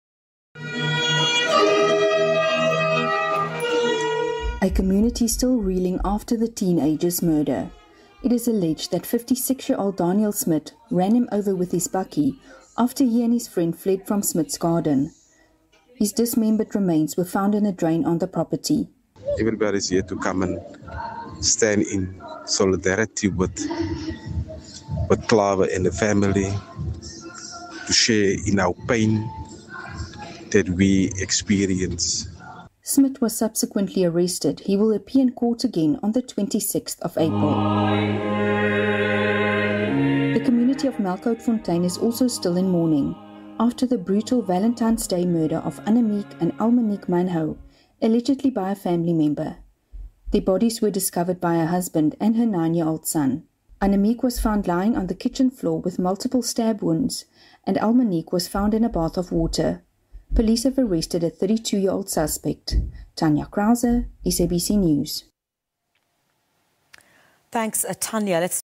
[WARNING: GRAPHIC CONTENT] 13-year-old Jerobijin van Wyk is laid to rest in Klawer
The community of Klawer on the Cape West Coast today gathered for the funeral of 13-year-old Jerobijin van Wyk. He was brutally murdered, allegedly by a 56-year old train driver, who apparently caught him and a friend stealing fruit from his garden.
Also today, 32-year-old Annemique and her two-year-old daughter, Almenique, were laid to rest at Melkhoutfontein near Still Bay in the Western Cape. They were killed in their home on Valentine's Day.
Please be warned, this story contains graphic details.